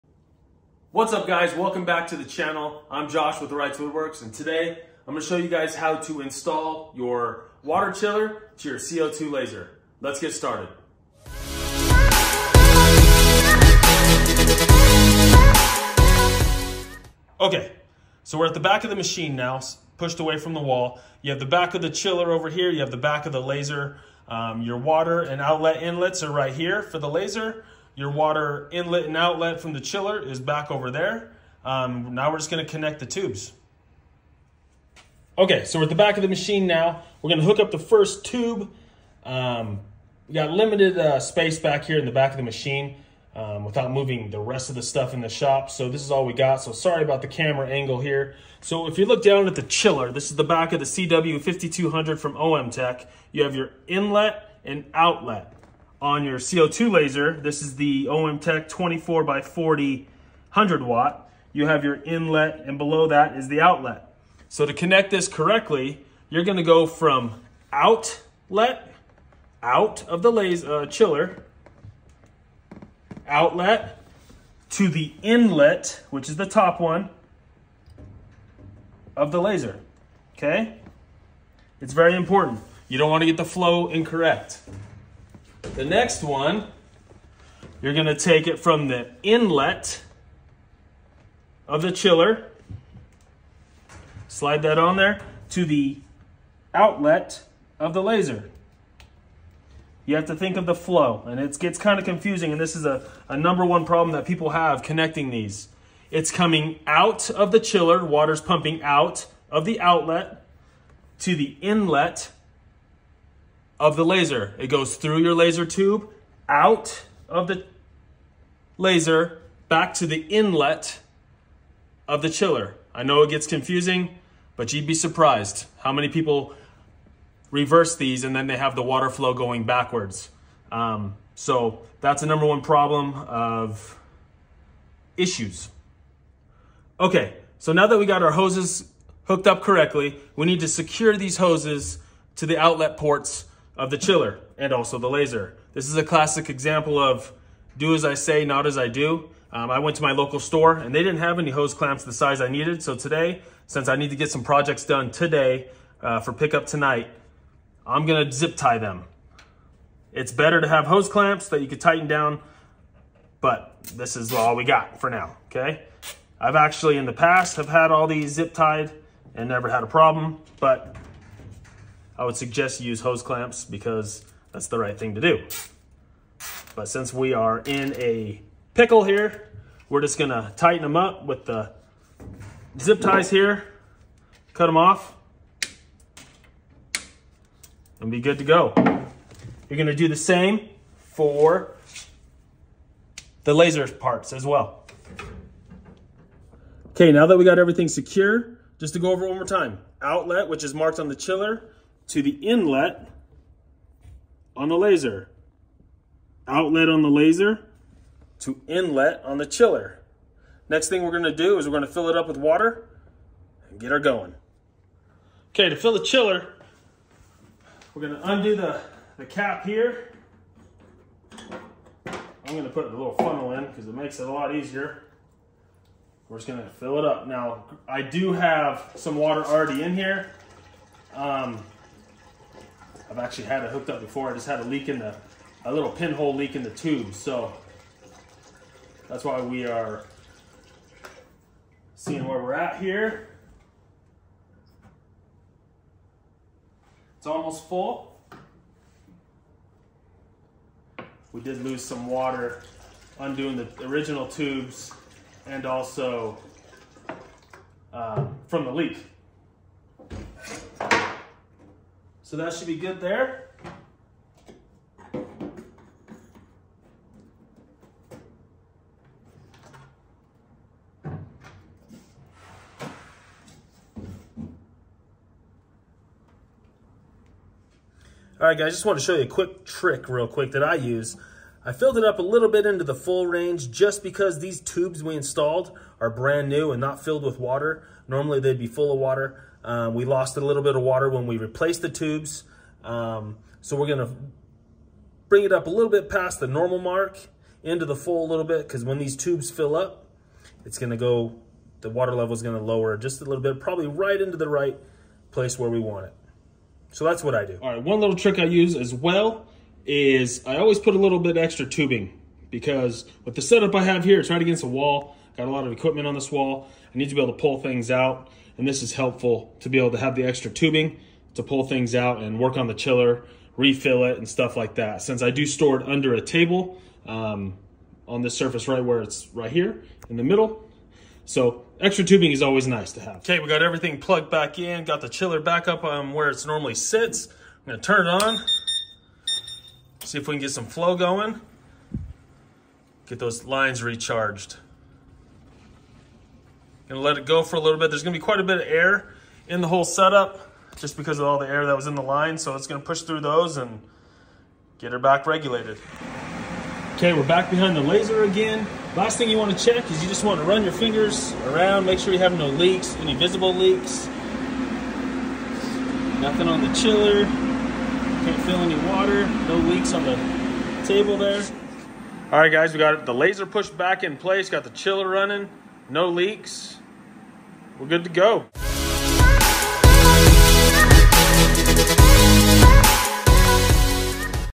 How to install/setup the OMTech Water Chiller to a CO2 Laser - Hose Replacement
A quick video showing the steps on how to set up an OMTech Water Chiller to a CO2 Laser.  If not installed correctly, the flow will be reversed, which could cause issues with the flow sensor, causing a alarm on the laser to sound.  Following these directions will insure you have the correct flow setup for your chiller and CO2 Laser.   Same steps are followed if you are replacing your water lines from the chiller to the CO2 Laser.

or
OMTECH CW-5200 Water Chiller (New)

Silicone Tubing (8mm ID/12mm OD)

Hose Clamps

OMTech Facebook Group: Come Join Us!